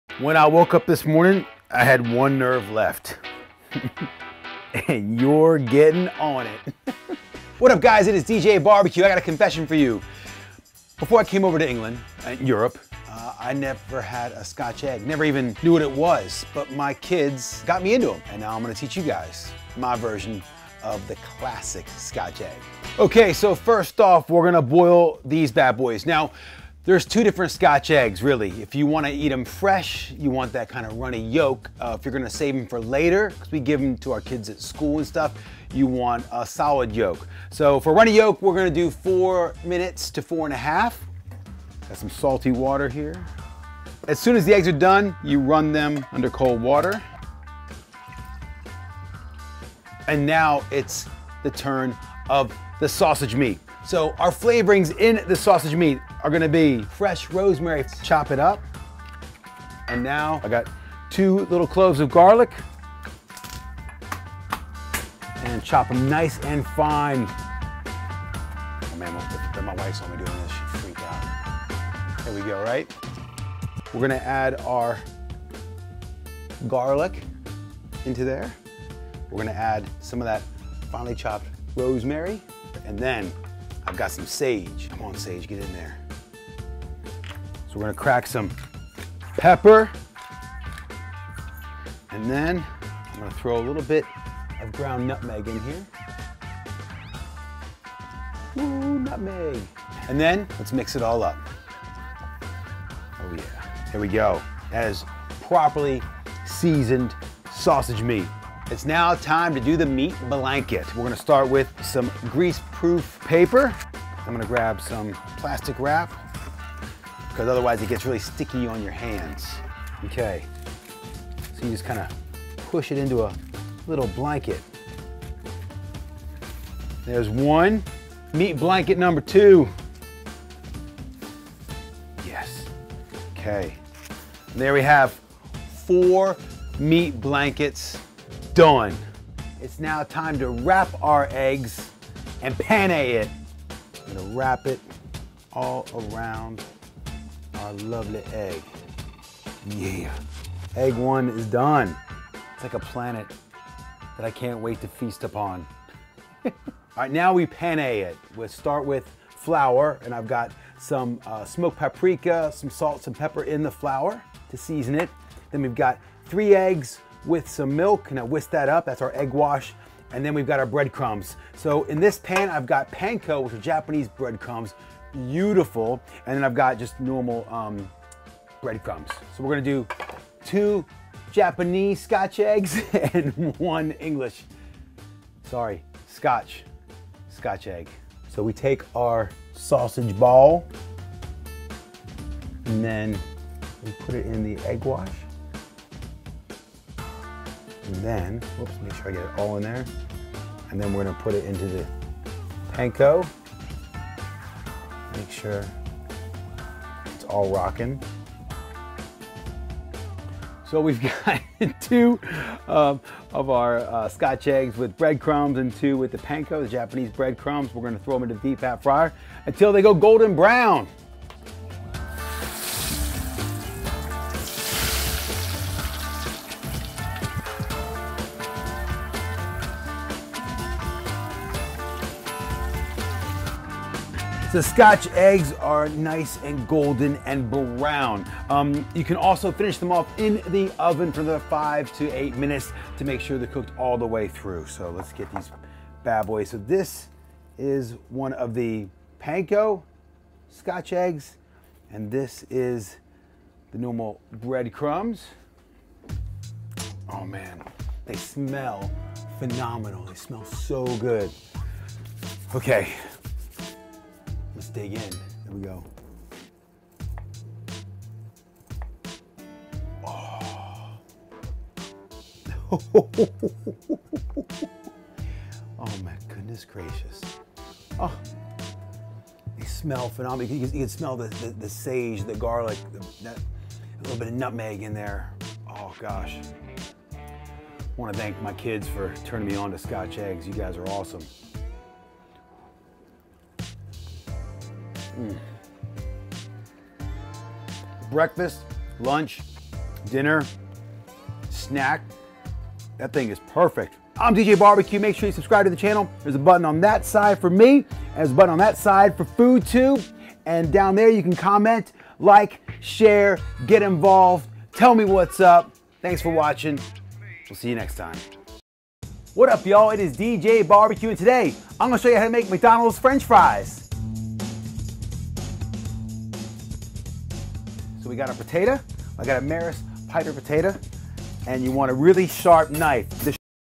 SCOTCH EGG
Love yourself this Valentine's and make these. What's better than an egg wrapped in meat and then deep fried? I dunno! Sounds like the best thing ever to me! Thank you Scotland for your meaty balls!